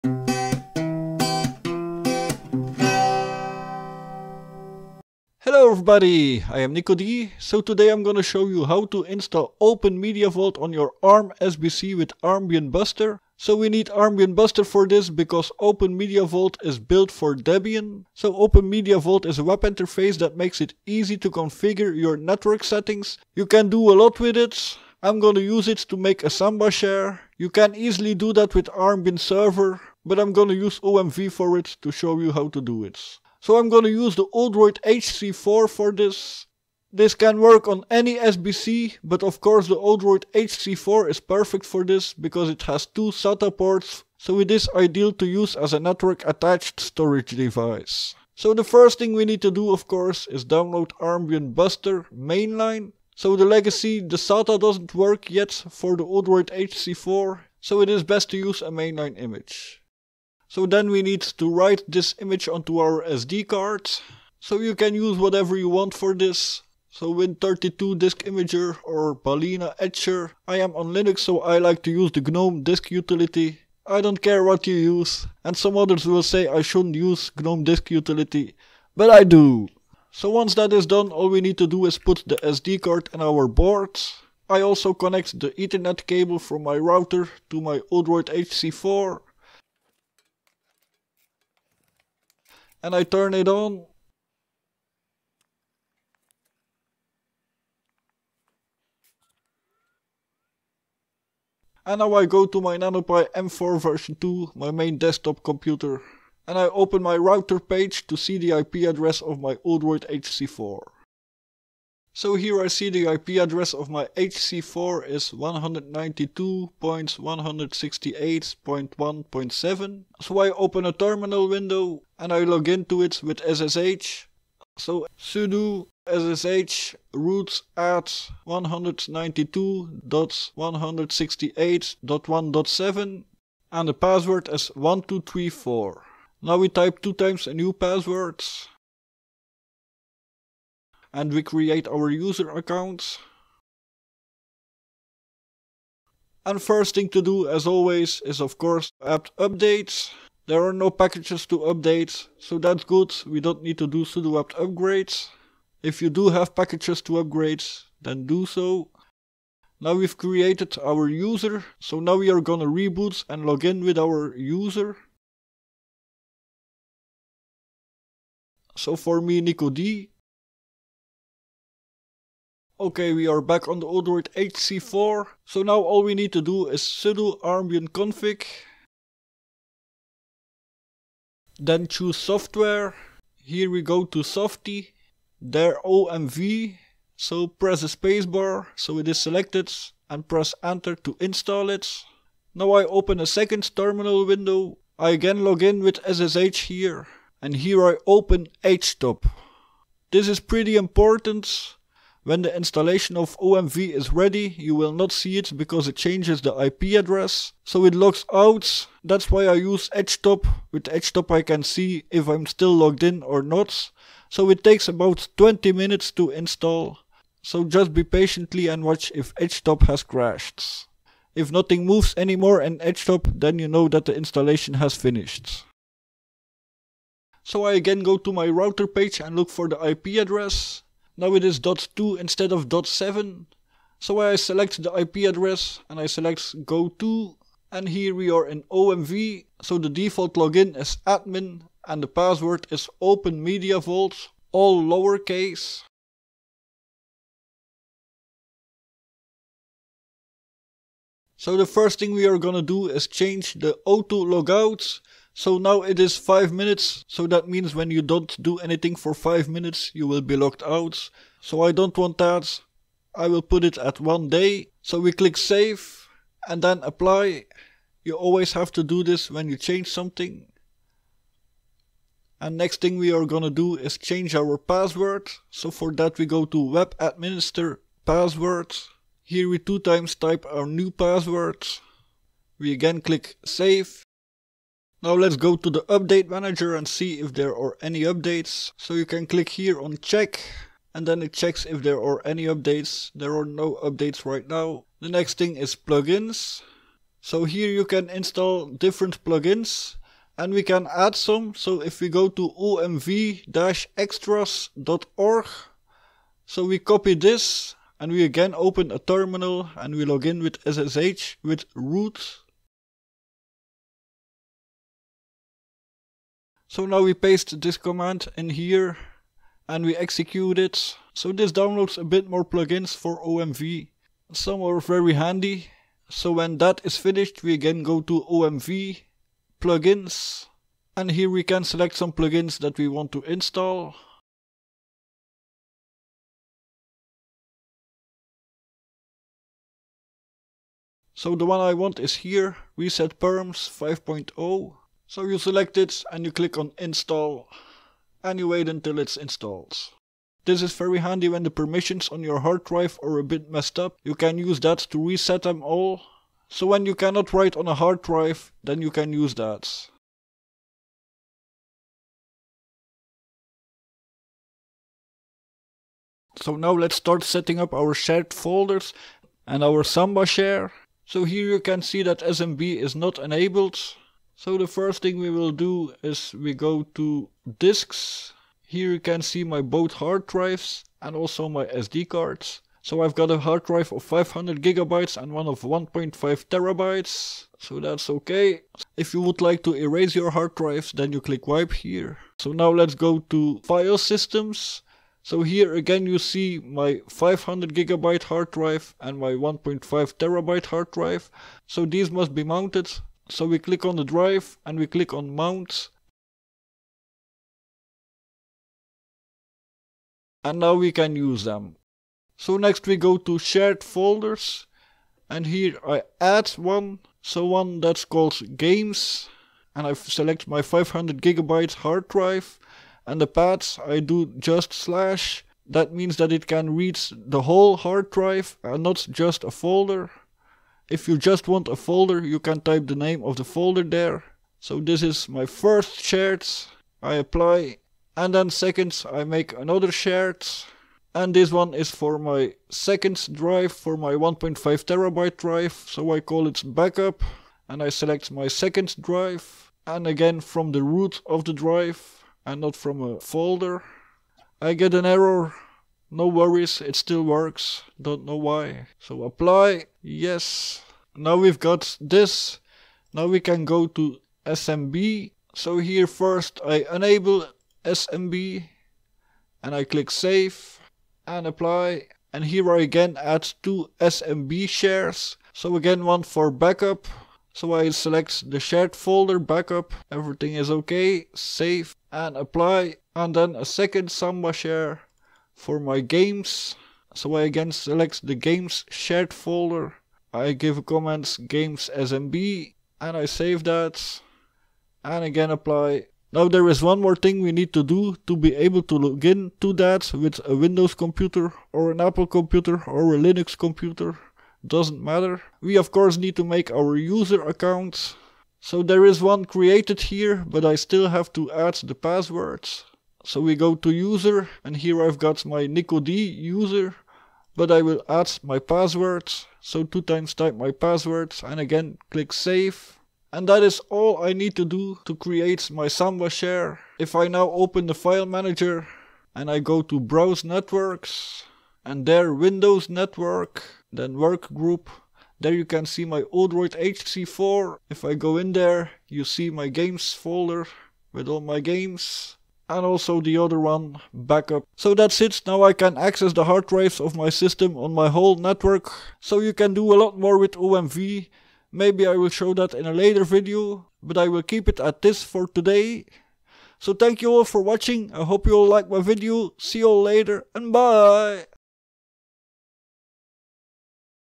How to Install OMV on your ARM SBC with Armbian Buster / Odroid HC4
How to Install OpenMediaVault on your ARM SBC with Armbian Buster